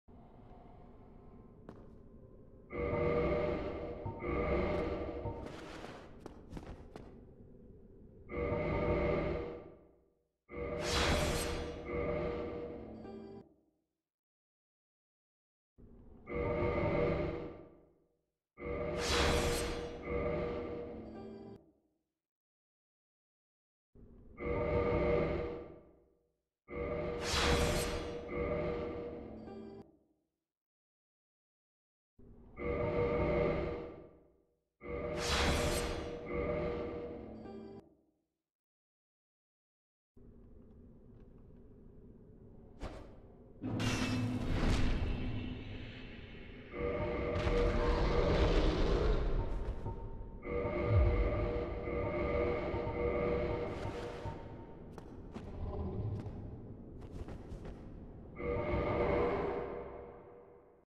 Firesage Elevator Clip
New skip  that saves roughly a minute in both Any% and All Bosses.